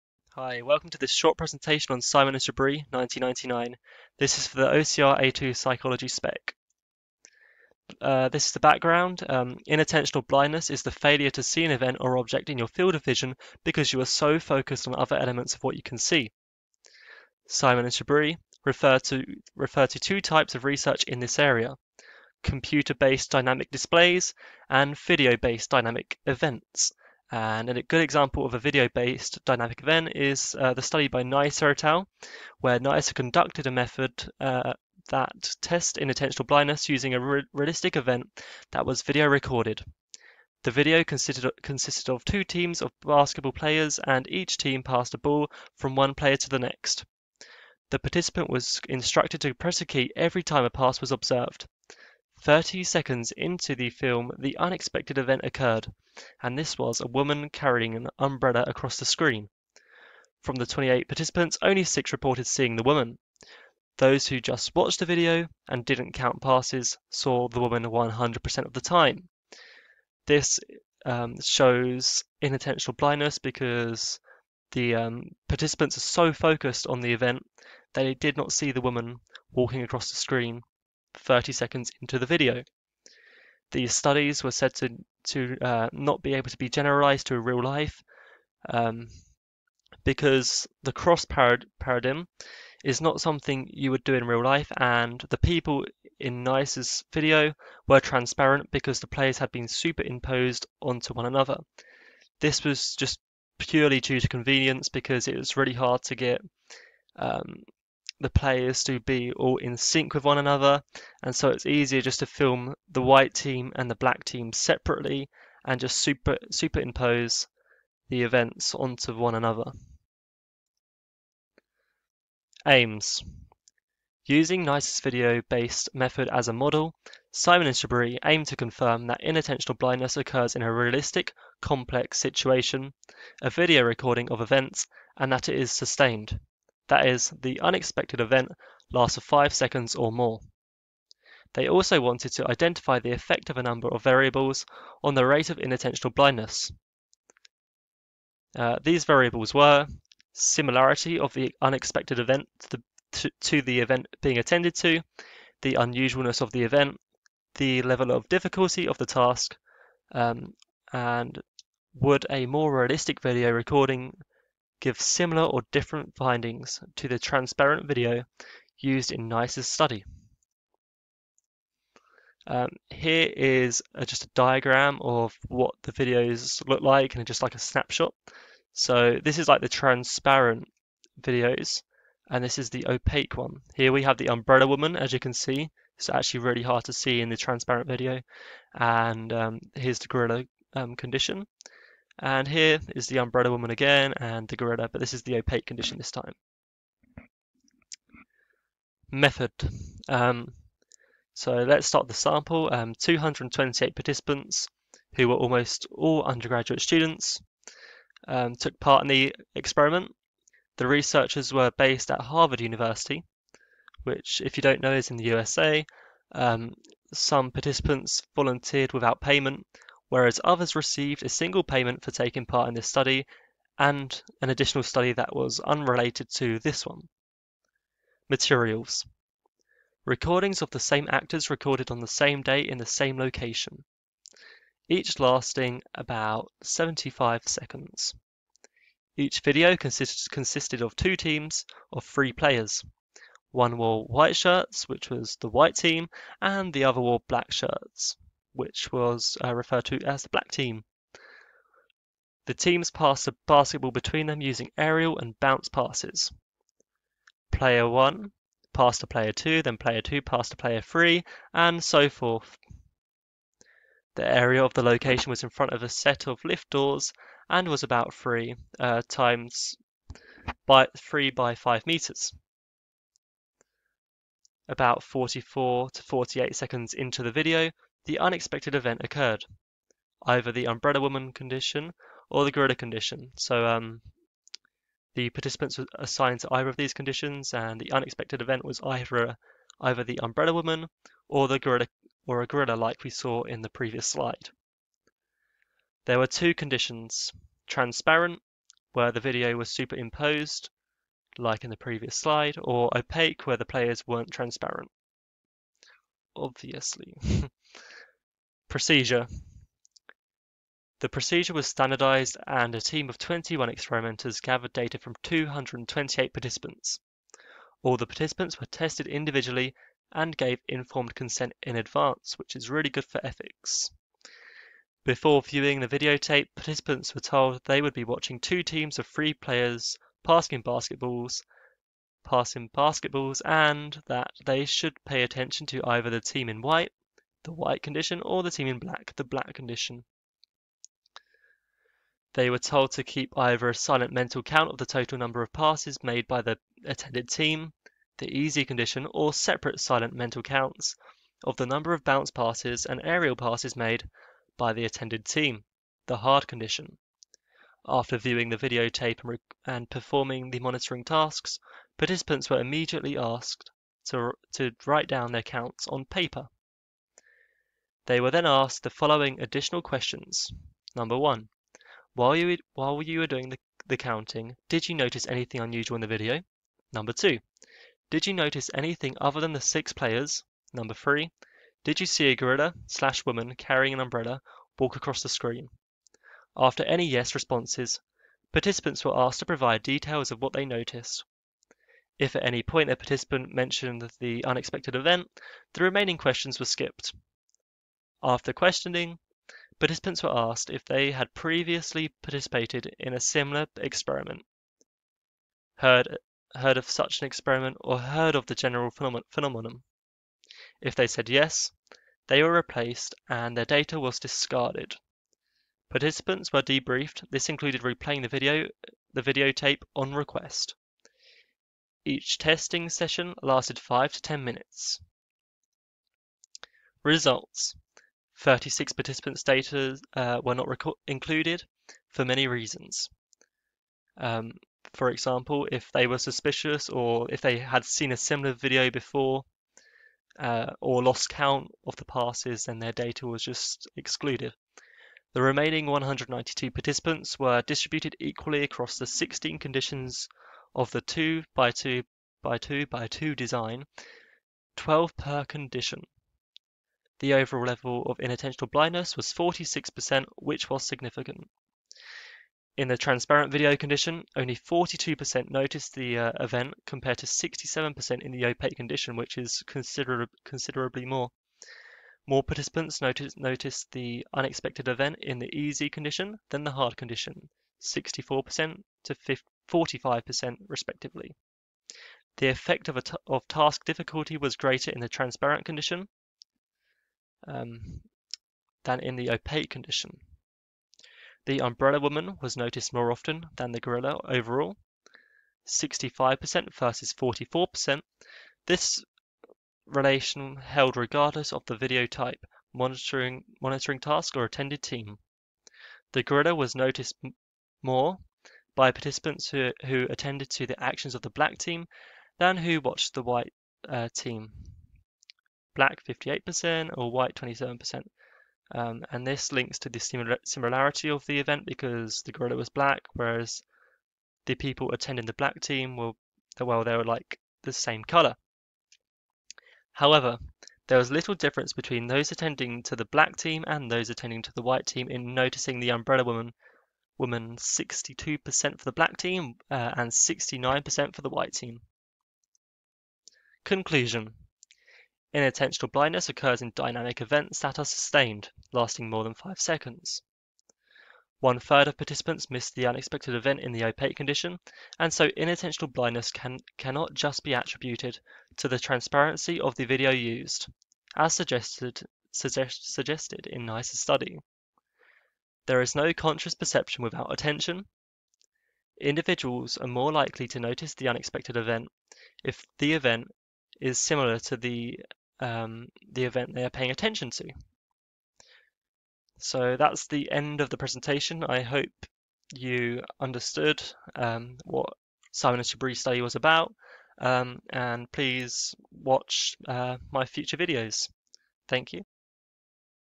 Simons and Chabris (1999)
Simons and Chabris (1999) for the OCR A2 Psychology Specification. Craeted using the OCR A2 Psychology Text Book (ISBN: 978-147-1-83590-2)